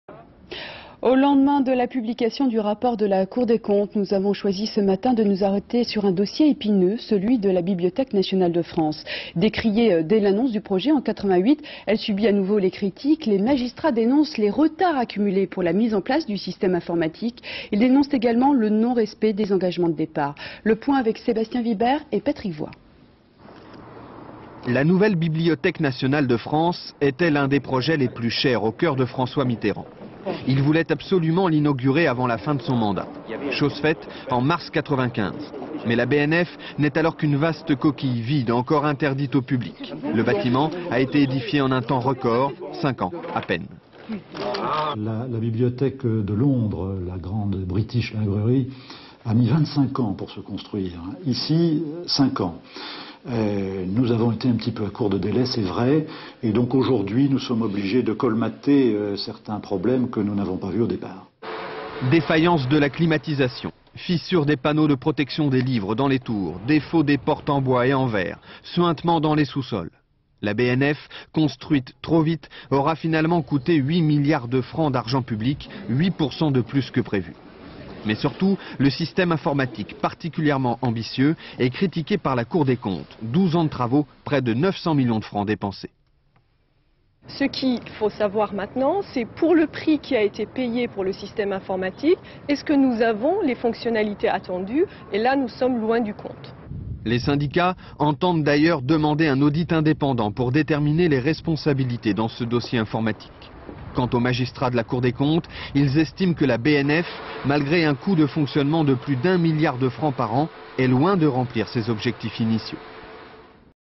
Rapport cour des comptes bnf
21 janvier 1999
La Cour des comptes dénonce dans son rapport annuel la gestion de plusieurs administrations et entreprises publiques et formule 21 observations nouvelles sur les incohérences, dérives et autres manquements de gestion de l'argent public. Dans son rapport 1998, livré mercredi 20 janvier 1999, la juridiction financière épingle notamment la Bibliothèque nationale de France (enveloppe financière de 7,9 milliards de francs - 1,2 milliard d'euros + dépassement annoncé du coût de un milliard de francs -152 millions d'euros).       - [Différents plans] Bibliothèque nationale de France : archives INA (inauguration, visite de François MITTERRAND).    - Interview François STASSE, directeur général de la BNF, qui évoque les cinq années de construction de la BNF contre les 25 ans qui ont été nécessaires en Grande-Bretagne pour construire la grande bibliothèque de Londres.      - [Différents plans extérieur] d'une des quatre tours de la BNF avec les arbres du jardin en contre-bas / [plan d'ensemble] escalier ouest / [gros plan] suintement et fuite d'eau dans les sous-sols de la BNF / [plan d'ensemble] couloir à moquette rouge / [panoramique] sur salle de lecture informatisée.       - Interview Anousheh KARVAR, militante CFDT, qui explique que les réalisations de la BNF ne sont pas à la mesure des ambitions initiales : "On est loin du compte au niveau fonctionnalités".       - [Panoramique] [contre-plongée] tours de la BNF.      - Groupe marchant sur l'esplanade de la BNF.      - [Plan d'ensemble] deux tours de la BNF. Images d'archive INA